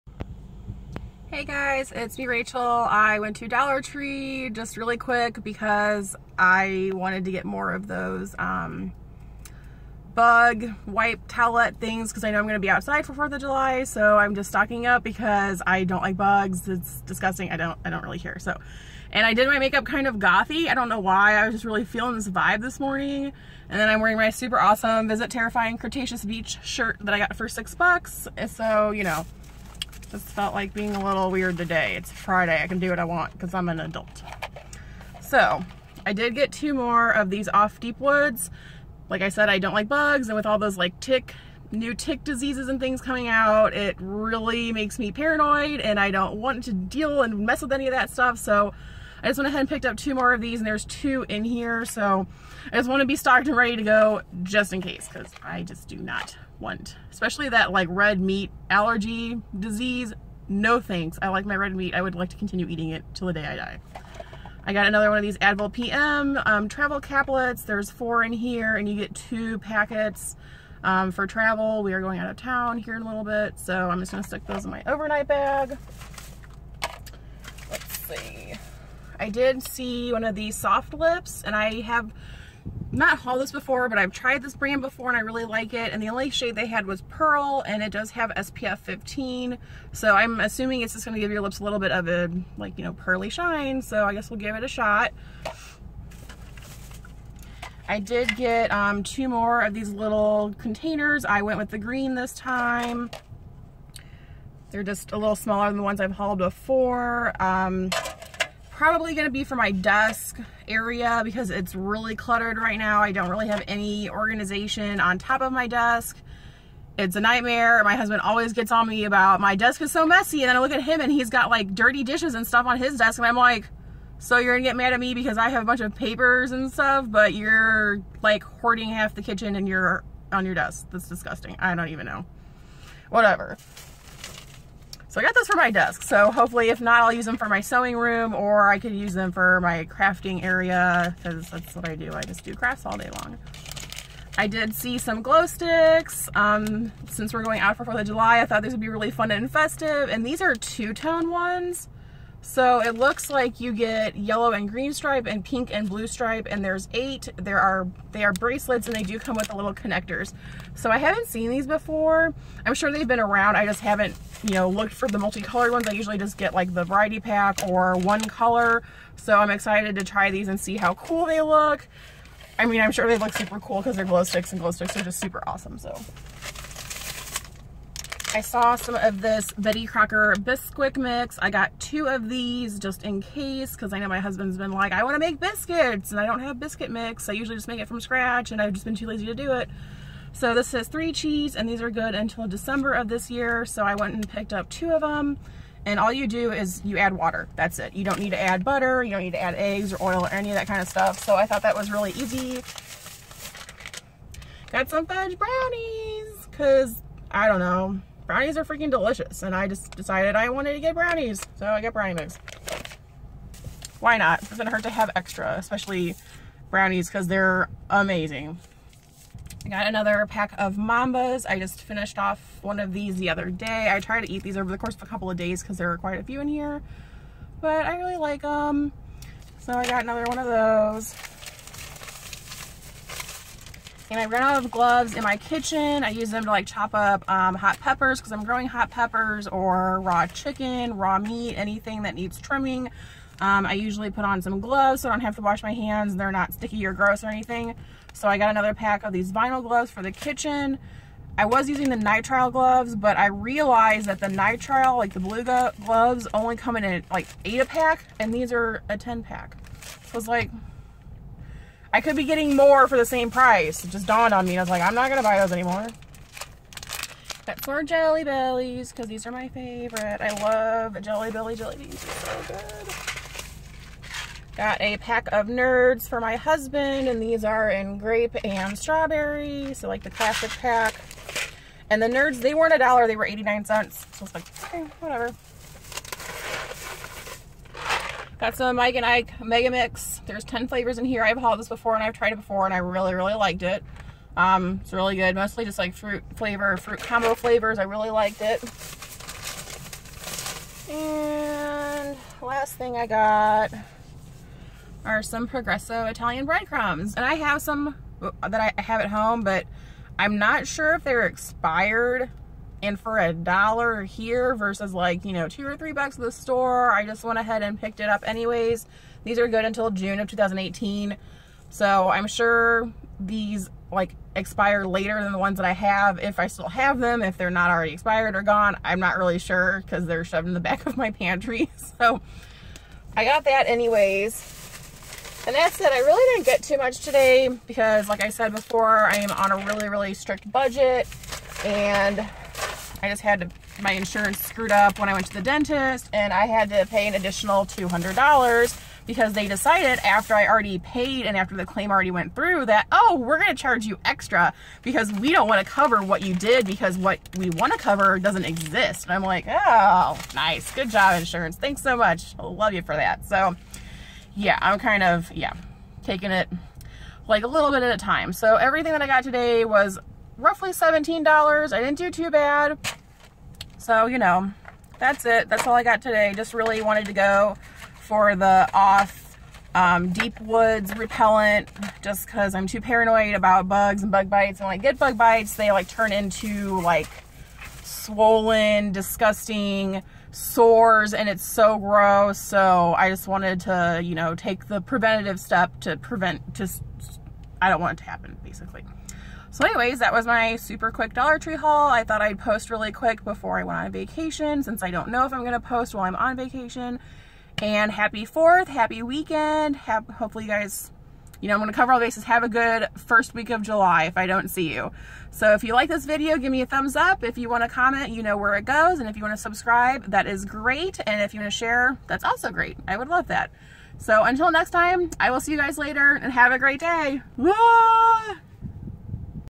Dollar Tree Haul 6/30/17
Quick Dollar Tree haul. I spent around $17. Hope you have a great weekend and have a safe fourth!